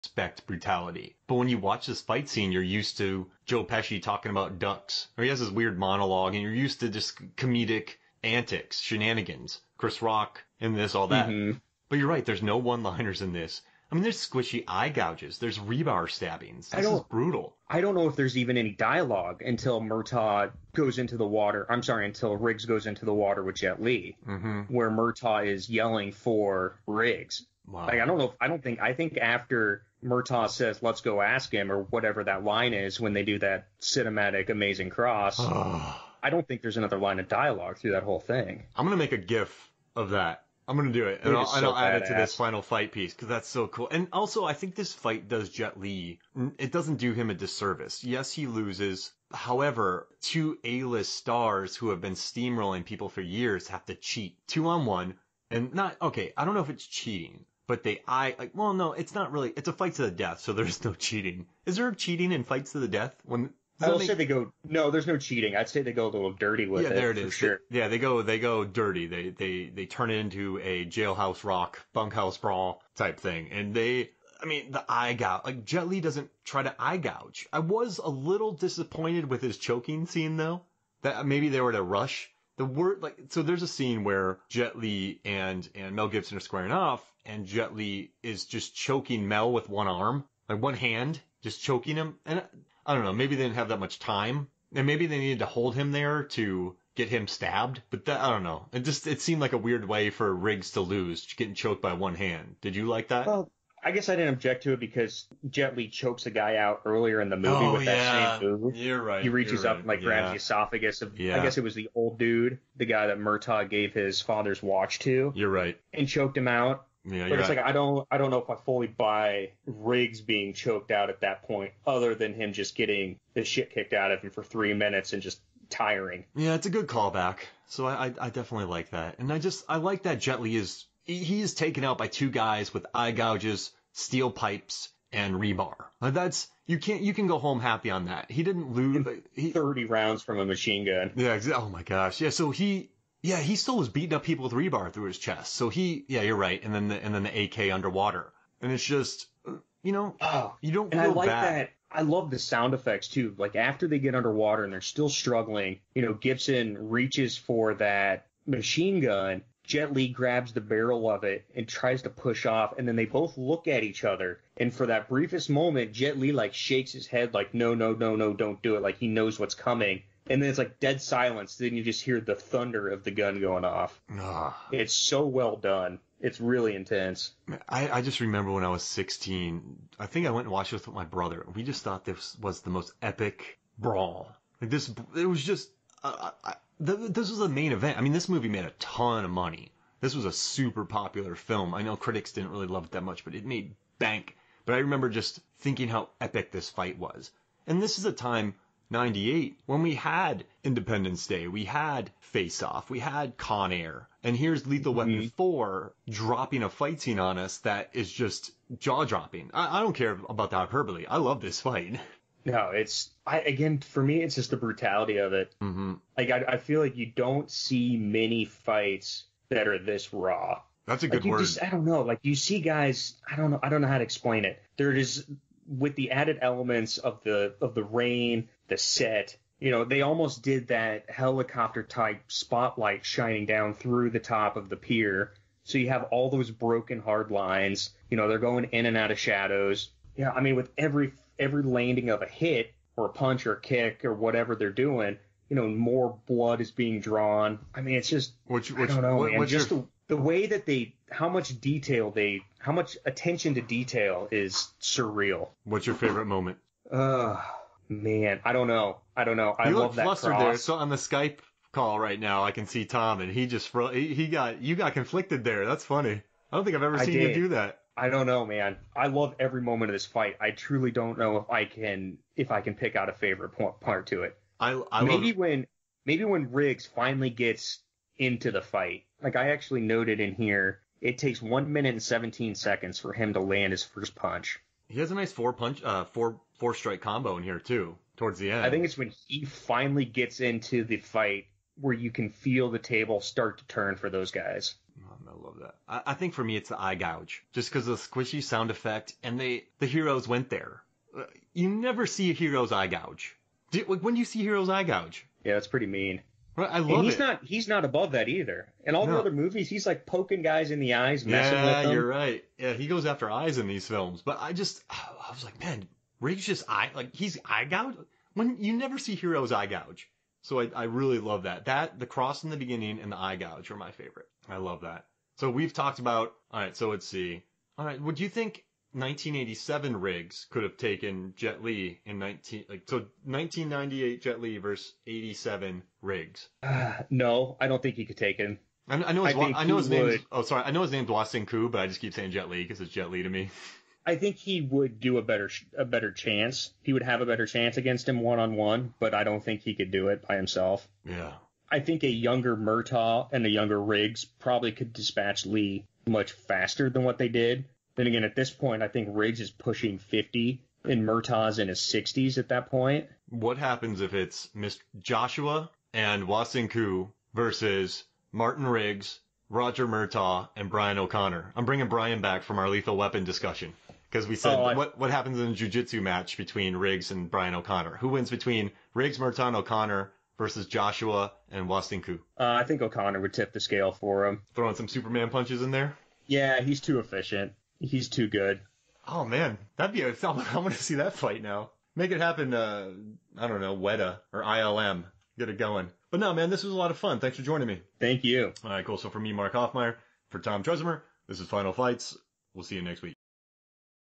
Final Fights - Episode 33 (Lethal Weapon 4 - Riggs & Murtaugh vs. Wah Sing Ku) (part 2 of 2)
Mark and Tom discuss the fun final fight between Martin Riggs (Mel Gibson), Roger Murtaugh (Danny Glover) and Wah Sing Ku (Jet Li) in Lethal Weapon 4. Enjoy!